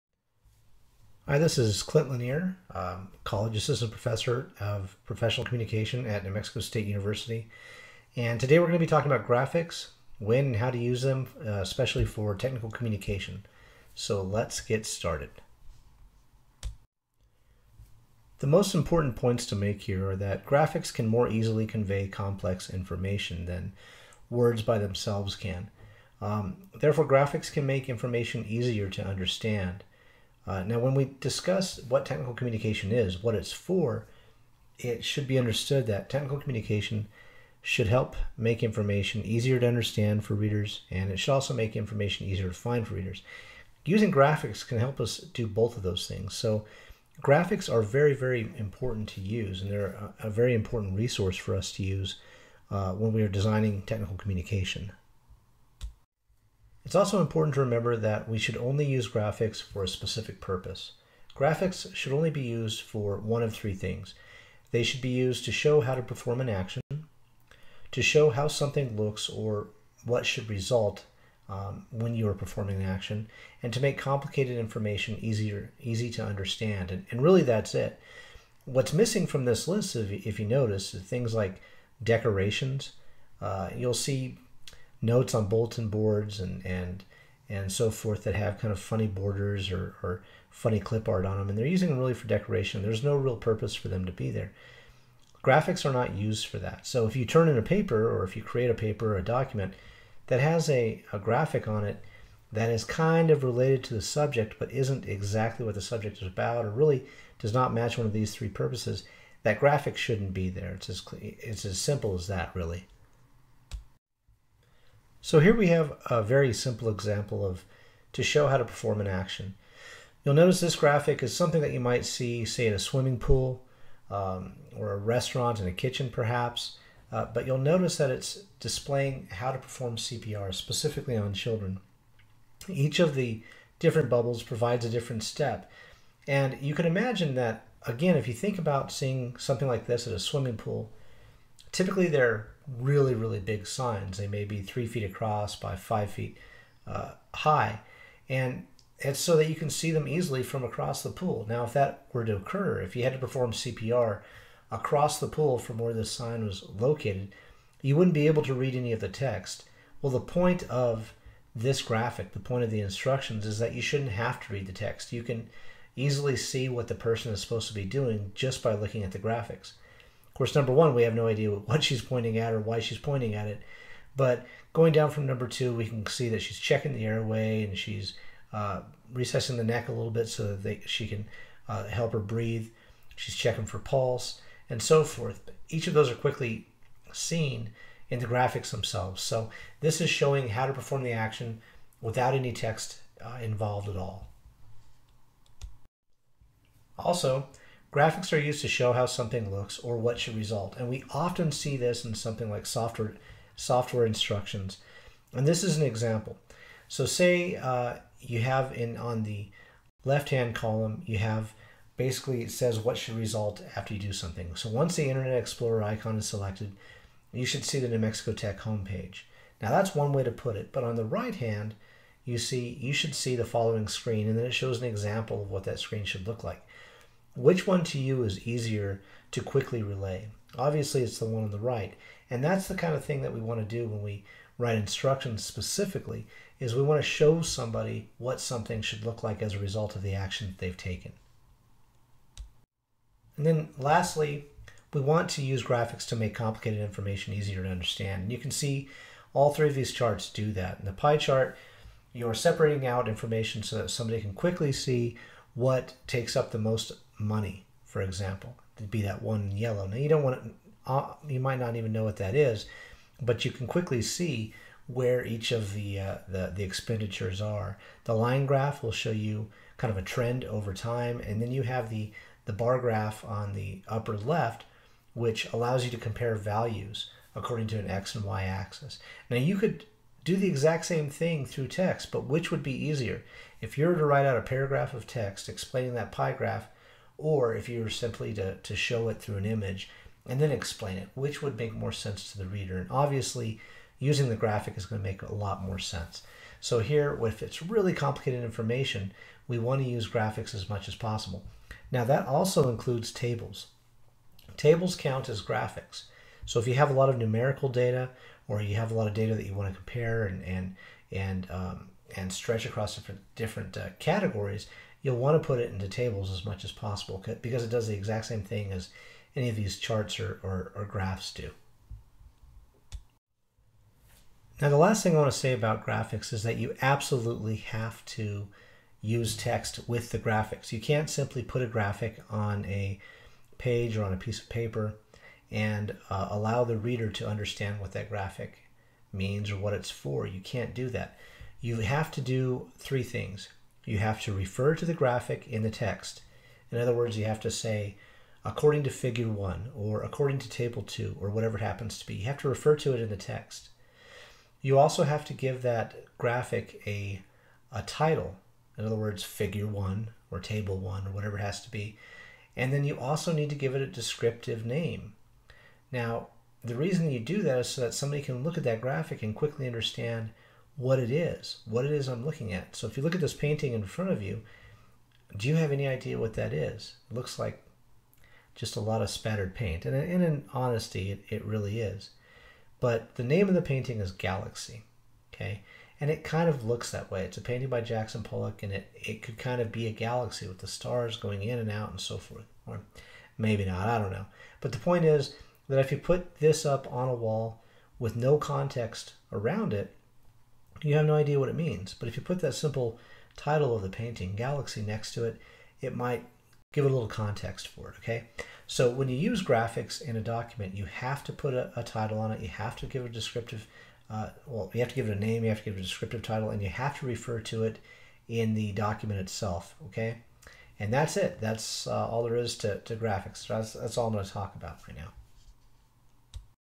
Using Graphics in Technical Documents
A very brief discussion about the use of graphics in technical documents, including why they are used and how to use them.